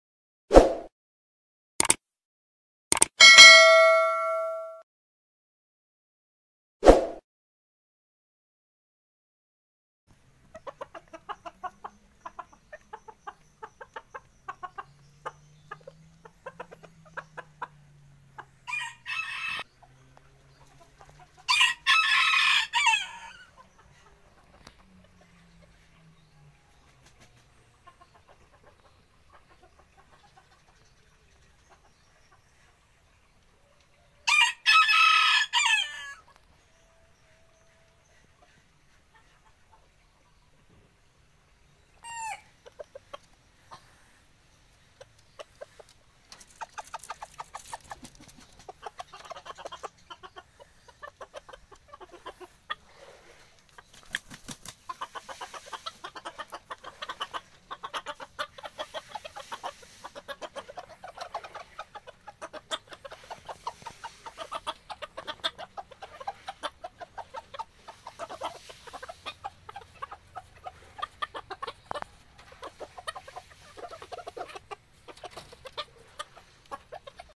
ngabuburit bareng hewan peliharaan‼️Ramadhan 2021
ramadan berkah ngabuburit sambil ngasih makan ayam dan ada juga yang lagi bertelur yaa